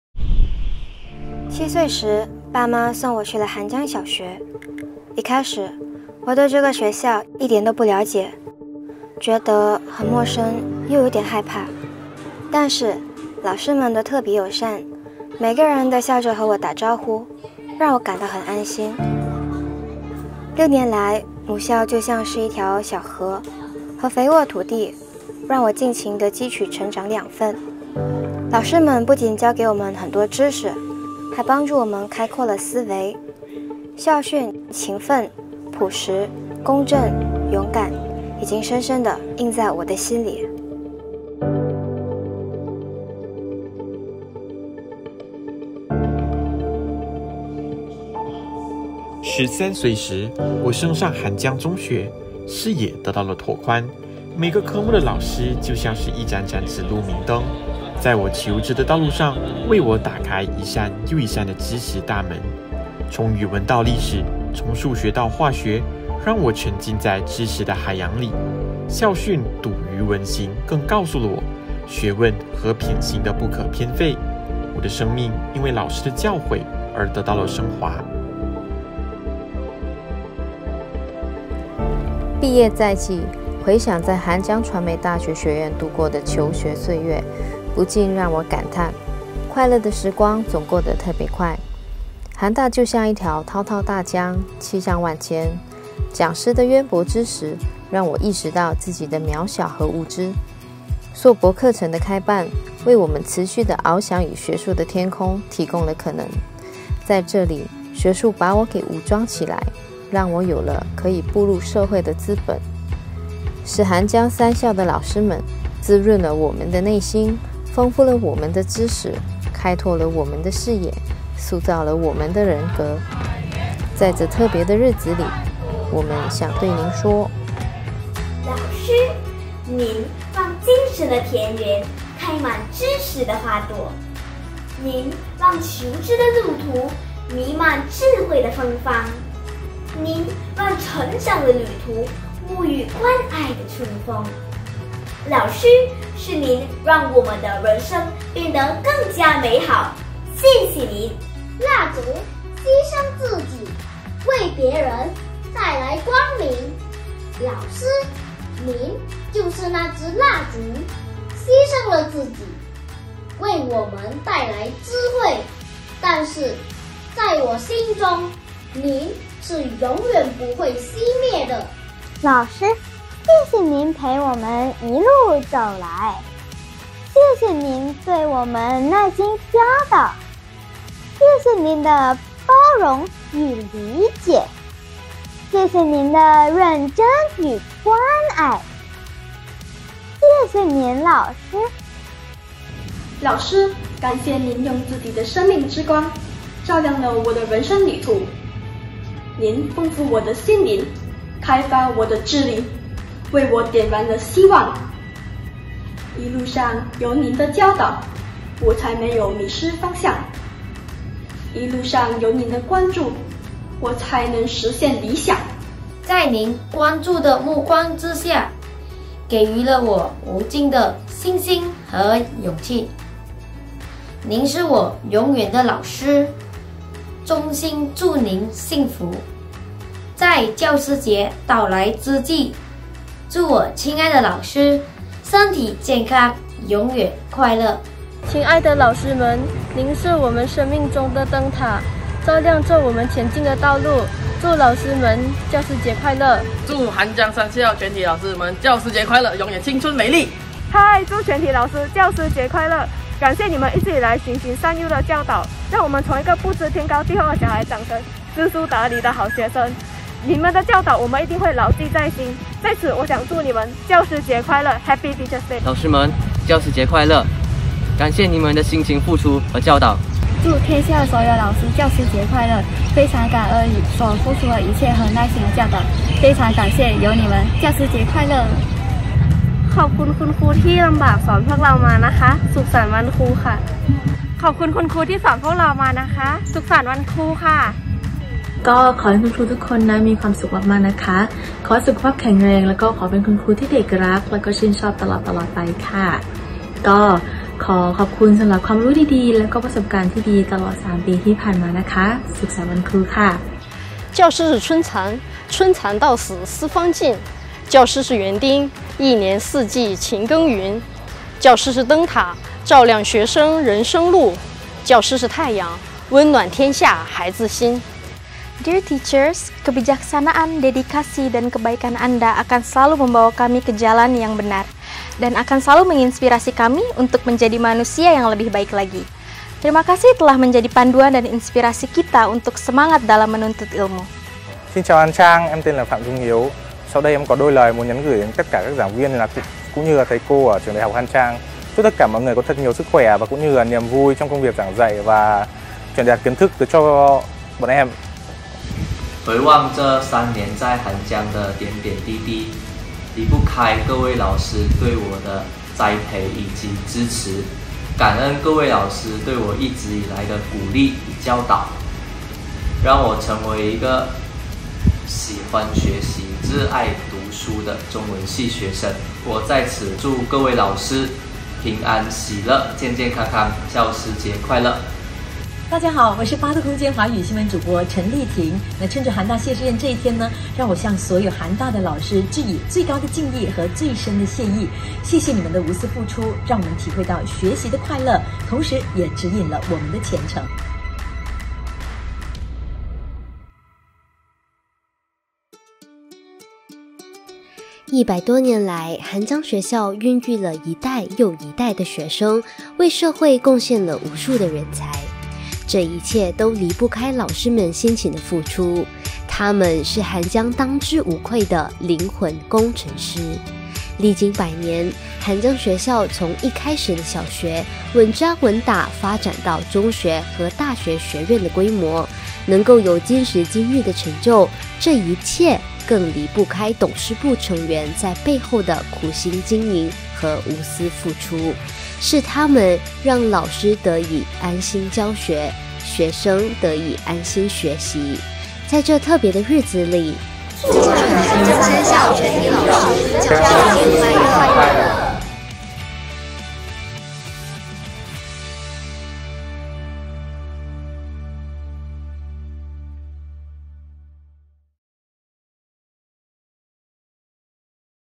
To all Han Chiang Teachers | 教师节特备
一百多年来，韩江学校孕育了一代又一代的学生，为社会贡献了无数的人才。这一切都离不开老师们辛勤的付出。他们是韩江当之无愧的灵魂工程师。

历经百年，韩江学校从一开始的小学，稳扎稳打发展到中学和大学学院的规模。能够有今时今日的成就，这一切，更离不开董事部成员在背后的苦心经营和无私付出。是他们，让老师得以安心教学，学生得以安心学习。

在这特别的日子里，祝韩江三校全体老师，教师节快乐！

For over a hundred years, Han Chiang schools have nurtured generations of students, cultivated countless talents. All these successes are inseparable from the unwavering dedication of the teachers. Teaching is a noble occupation.

Throughout the ages, Han Chiang started its inception as a primary school and later expanded its institution to a secondary school and university college.

To be able to have today's achievements. All of this, and even more so, is deeply indebted to the board members' diligent management and selfless dedication.

They provide a conducive environment for teachers to teach without worry and create a supportive environment for students to learn.

On this special occasion, we wish all the teachers of Han Chiang a Happy Teachers' Day!
欲知更多资讯，欢迎浏览我们的页面